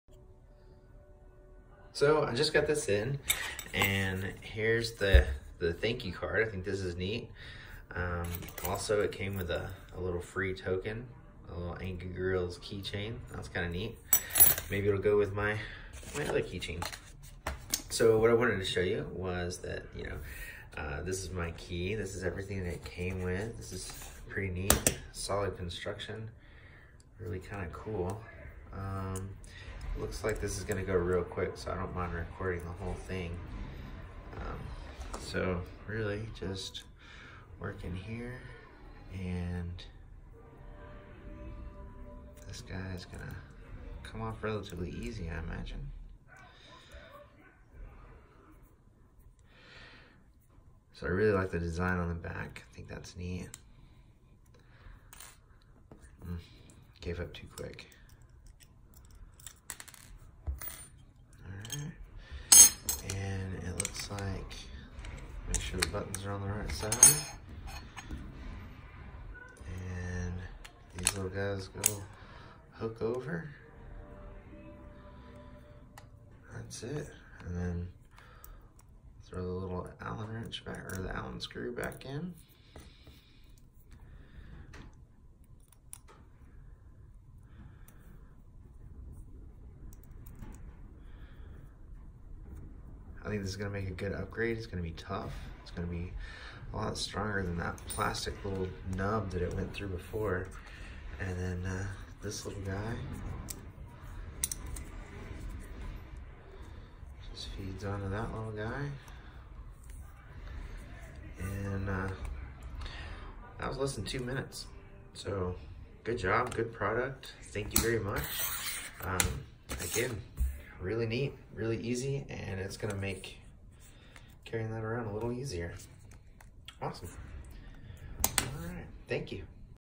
Jeep people doing Jeep things! Key Fob Review
Installed the metal key fob protector! Looks great very strong!
JeCar Metal Remote Key Fob Cover...